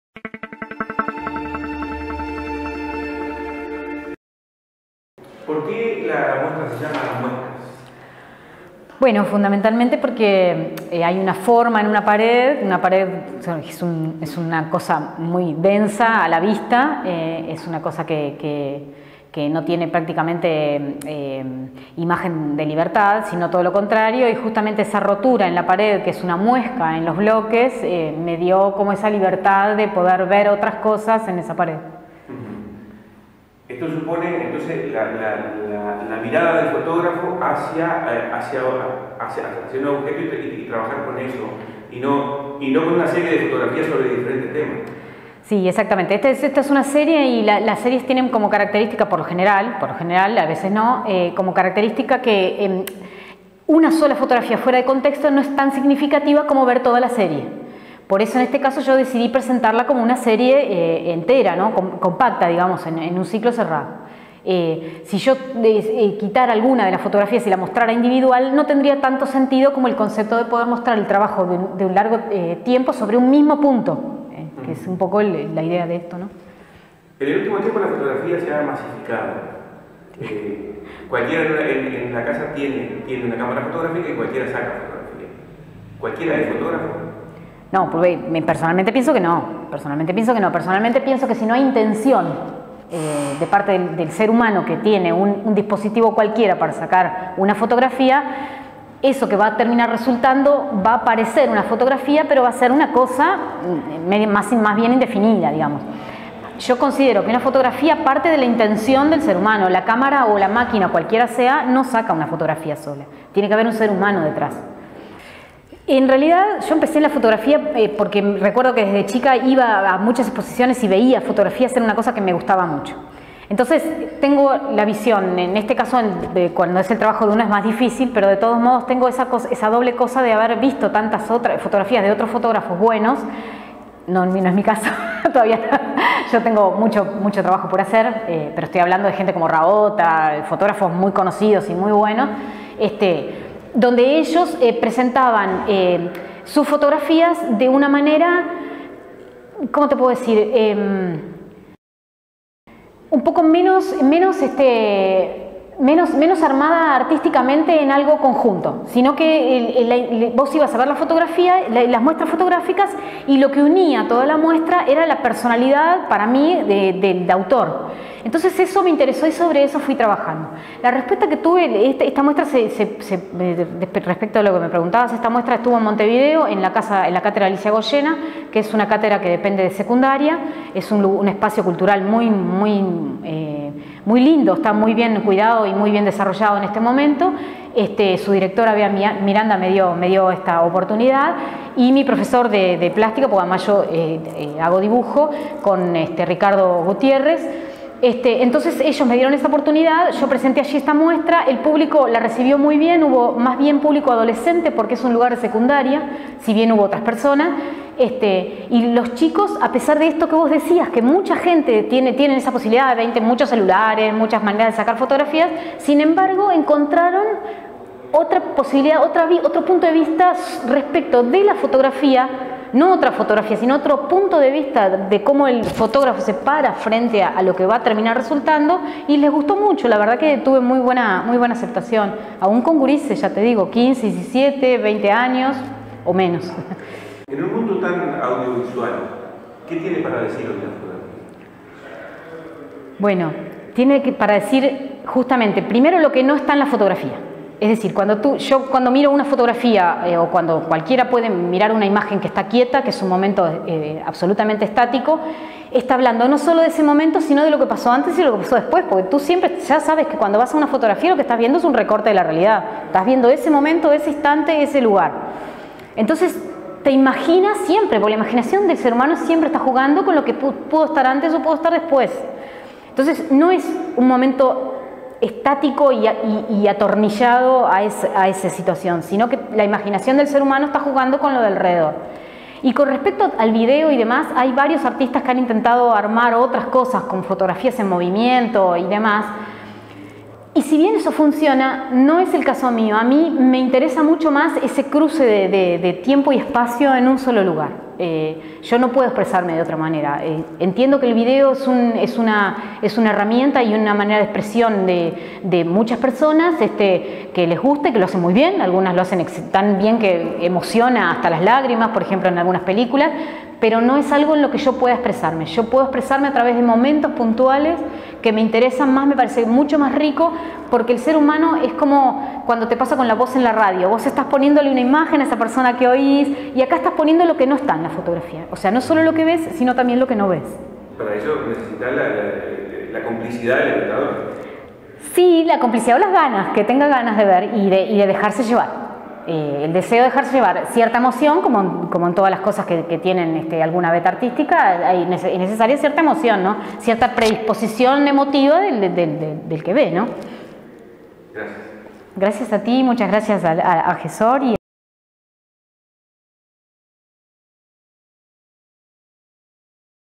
vilma Bada
Dialogamos  con   la   fotógrafa    Vilma Bada sobre   su exposición   "Muesca"  que  se  está presentando  en Casa Puerta de   Mercedes  hasta  el  22  de  setiembre 2016.